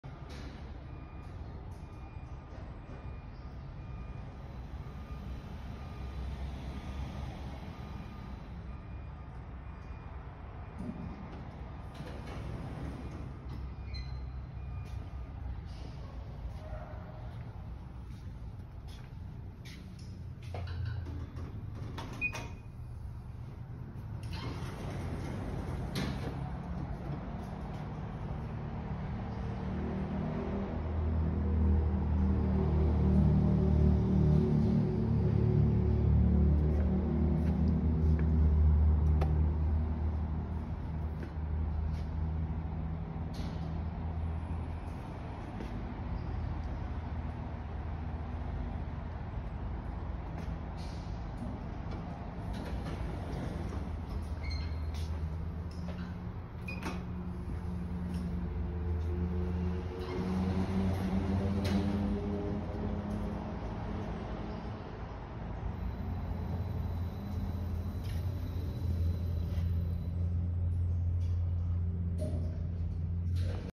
Changing speeds for Levelling at short floors and different heights Glass lift elevator Sydney
Ecolift installed Spanish Sumasa lift and elevator with Carlos Silva controller and Fuji frenic drive changes speed depending on the calculated floor distance and required stopping lengths